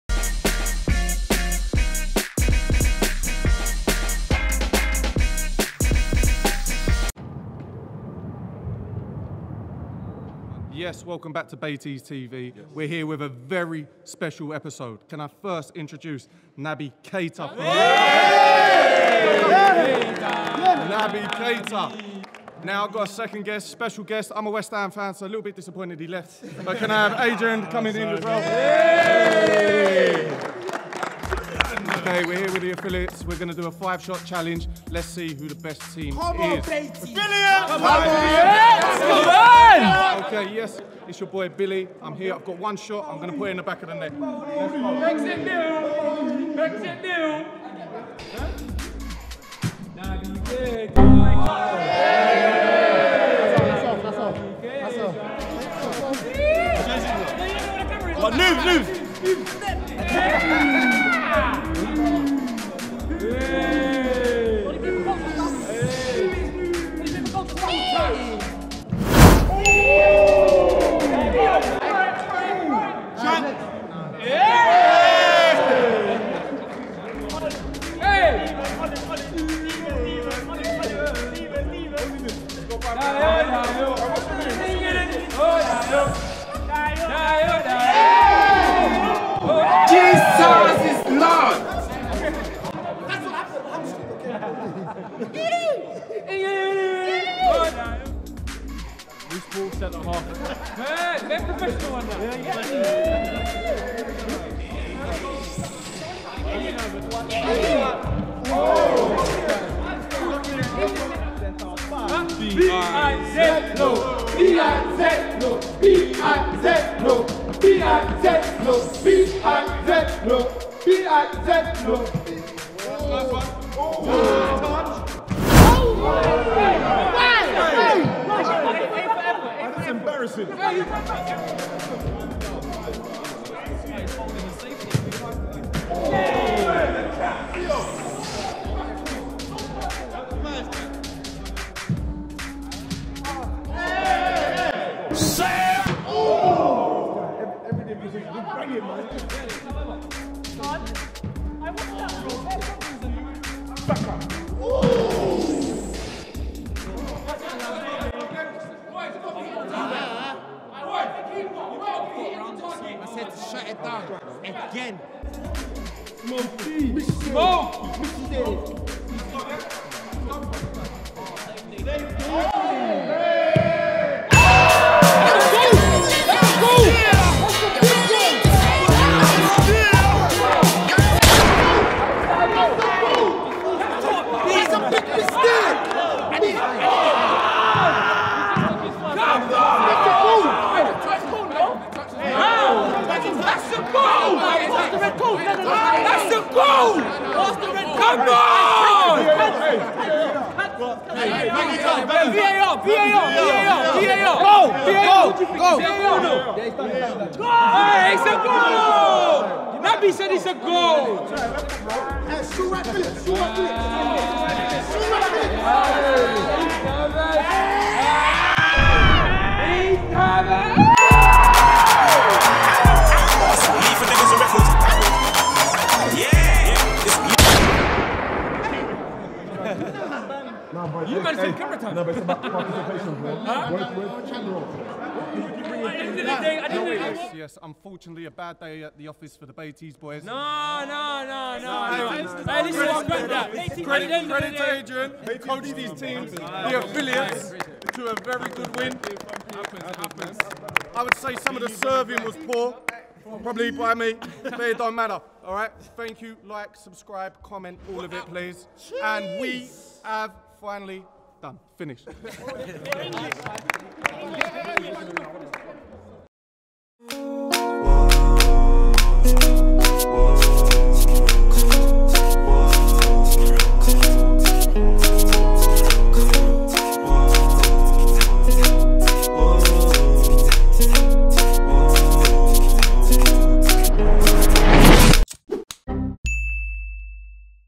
BAITEZE VS YUNG FILLY’S AFILLYATES | TWO TOUCH CHALLENGE
Welcome back to Baiteze TV (liking the consistency ay👀, you speak we listen!)
In today’s episode we have Yung Fily’s Afillyates taken on the Baiteze Two Touch Challenge @ thé Liverpool training ground with Mane, Chamberlain, Gomez, Adrian & Keita all present to see how it unfolds!